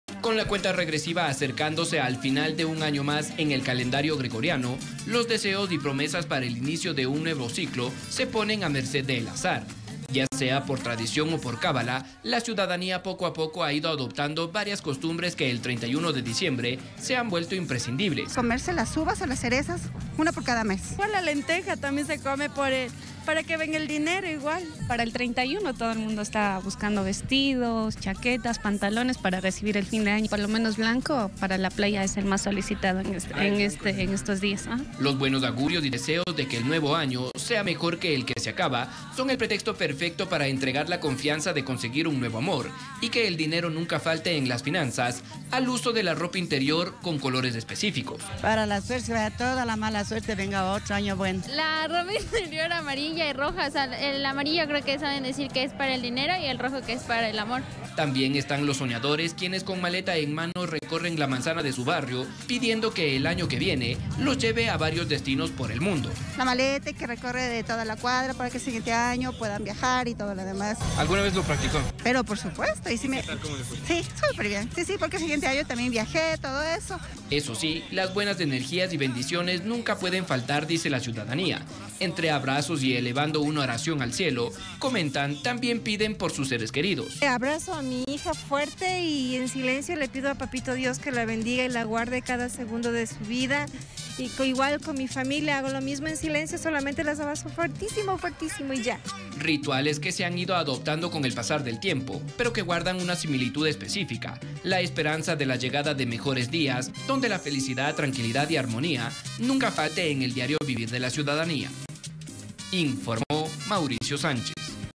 Ciudadanía prepara las cábalas para recibir el nuevo año 2023
Para la salud, el dinero y el amor: la población nos cuenta cuáles son sus mejores rituales.